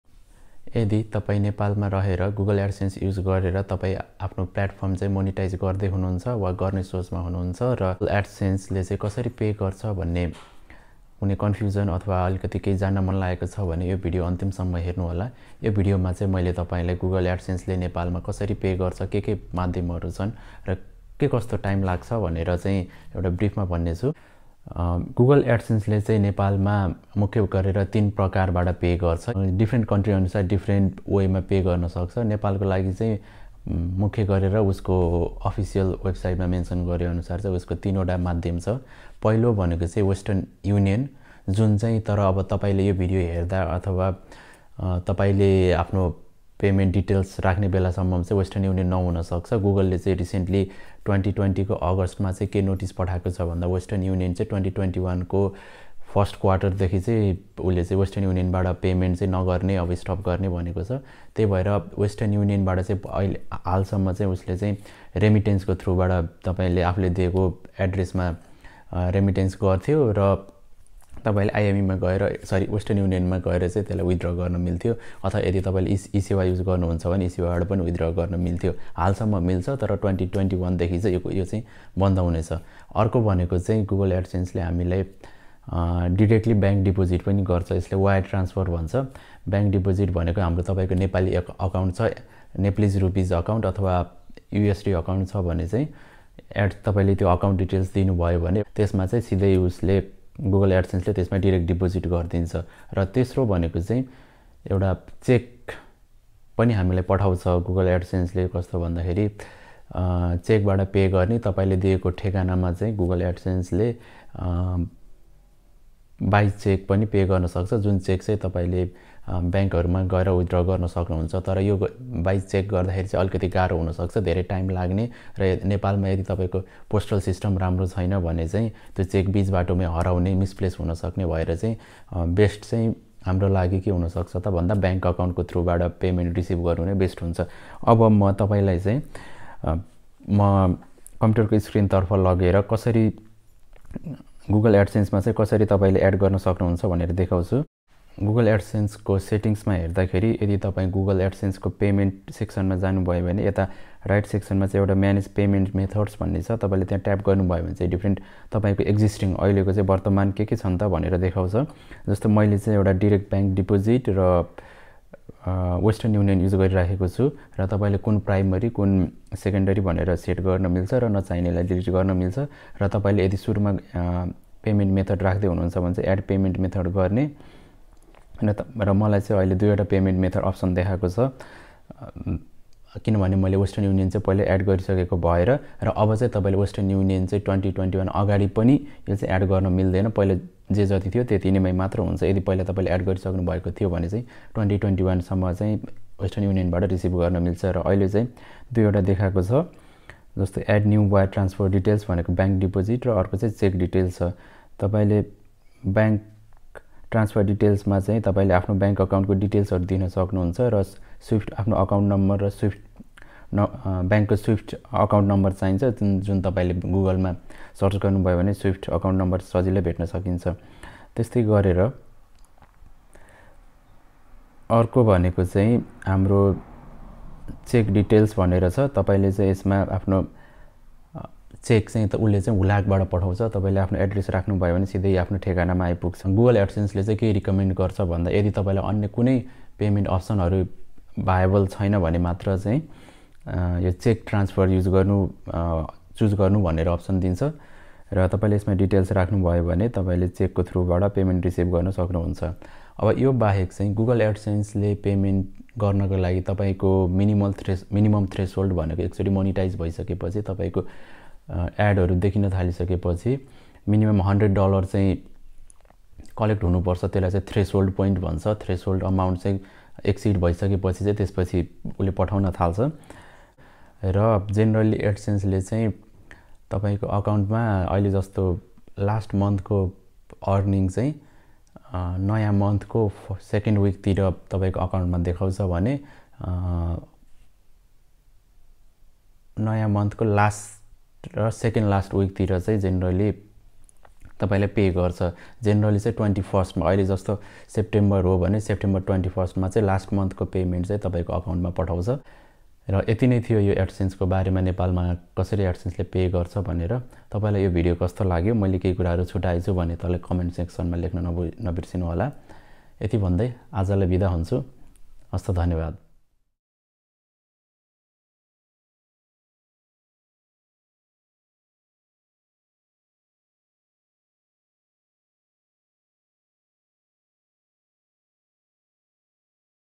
How does Google Adsense Pay in Nepal? 2022 Methods, Setup & Schedule
A short video explaining and demonstrating how Google Adsense pays publishers in Nepal and the different available Google Adsense payment methods for publishers in Nepal with a live demo.

Western Union will no longer be available when adding your form of payments

This Nepali tech video will help you on:
- How to withdraw from Google Adsense in Nepal
- How does adsense pay in Nepal
- Things you need to know about AdSense's wire transfer/ bank transfer method
- quick update about the payment withdrawal through Western Union and when Google is stopping to pay publishers through western union remit
- payment by check
- payment schedule of adsense in Nepal

Keep in mind that we may receive commissions when you click our links and make purchases.